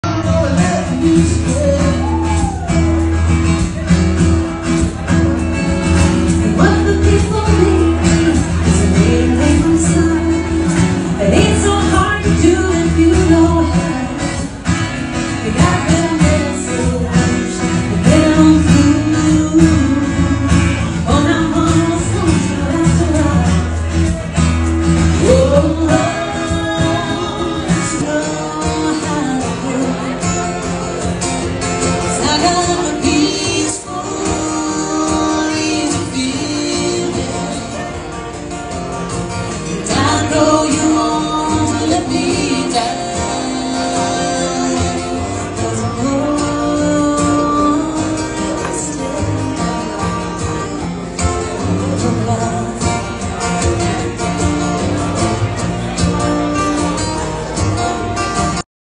Live Music! 🎶Radio Farm 🎶 at Puckett’s Nashville
We loved the LIVE MUSIC!! Thank you to RadioFarm at Pucketts -- NASHVILLE dinner and a SHOW !